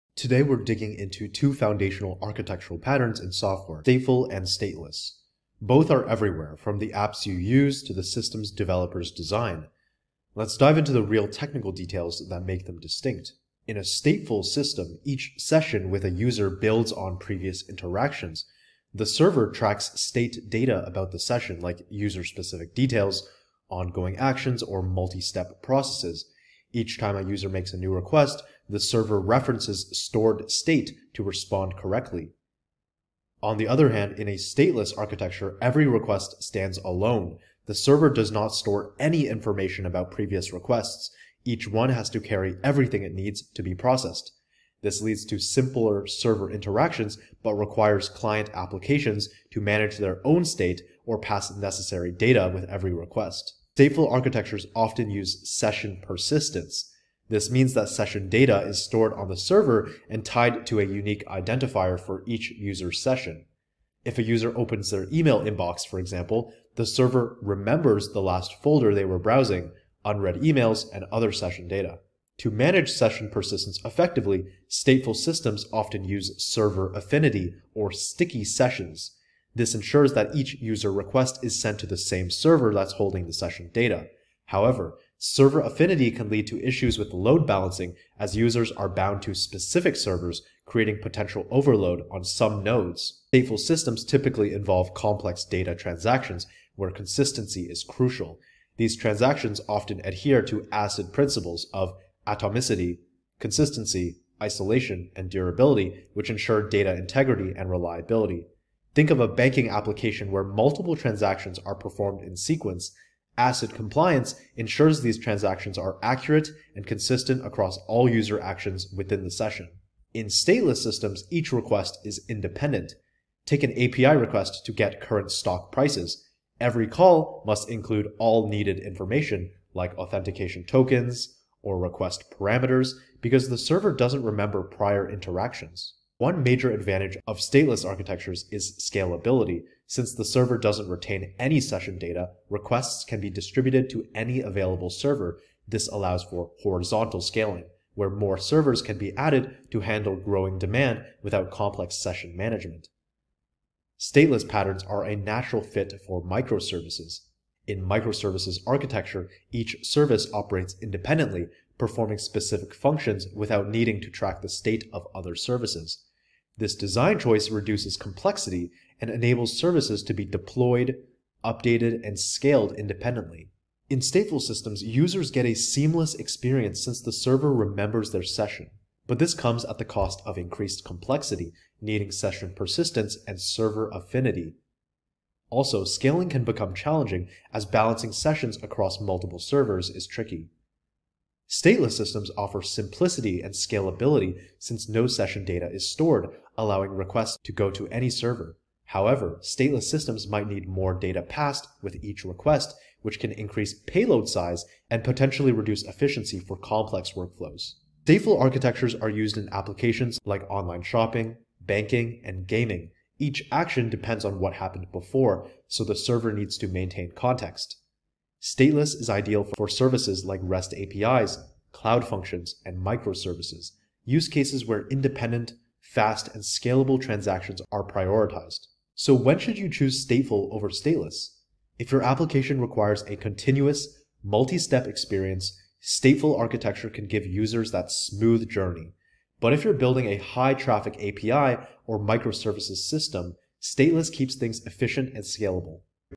Stateful vs Stateless: Which One Do YOU Need?
In this video, we dive into the key differences between stateful and stateless architectures, two foundational concepts in web development and software design. You'll learn how each architecture manages data, handles requests, and serves clients differently.

📌 We'll cover:

- What is stateful architecture? How it maintains session data and why it's essential for certain apps.
- What is stateless architecture? How it scales efficiently and why it's used in microservices and REST APIs.
- Real-world examples to see these architectures in action.
- Key advantages and disadvantages of each.